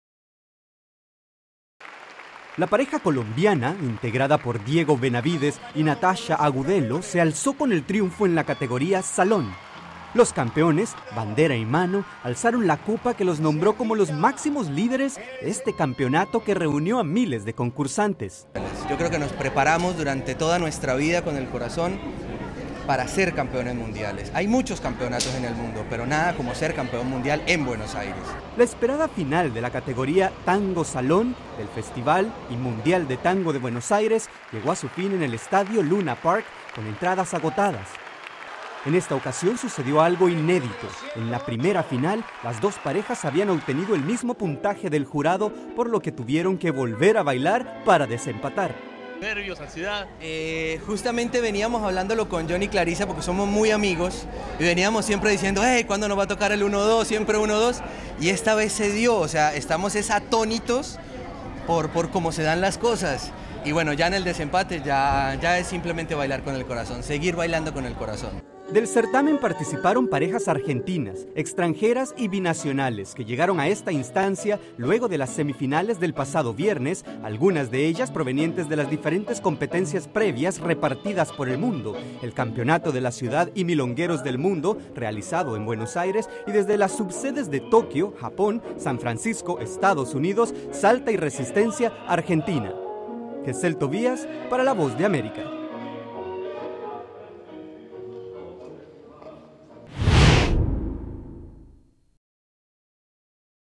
Colombianos triunfan en mundial de Tango
Los rioplantenses fueron desplazados por la pareja de colombianos liderada por Diego Benavídez en el campeonato mundial de Tango de Argentina.